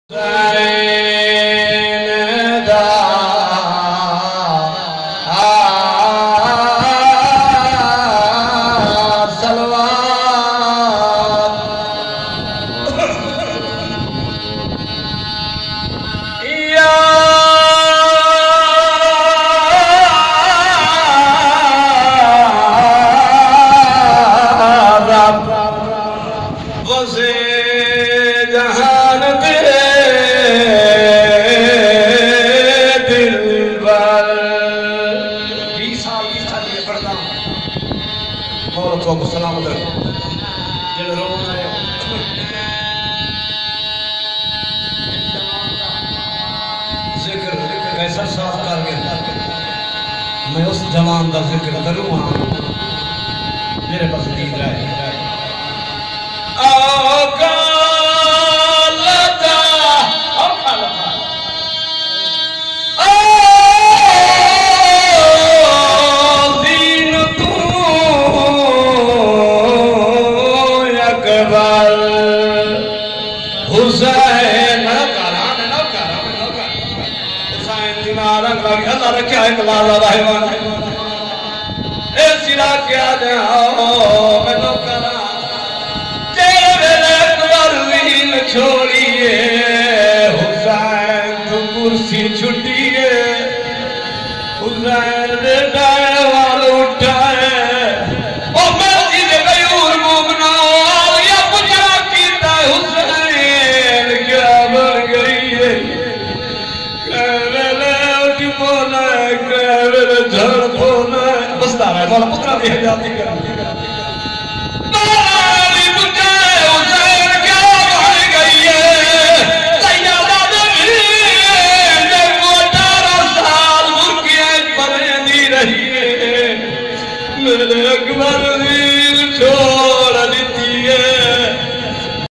Zakir Gulam Hussain Shershah 22 Ramzan Qadimi Majlis e Aza Mailsi Fadda Town
Bani Majlis e Aza Zawar Malik Maqbool Hussain Zargar
Zawar Aamir Raza Advocate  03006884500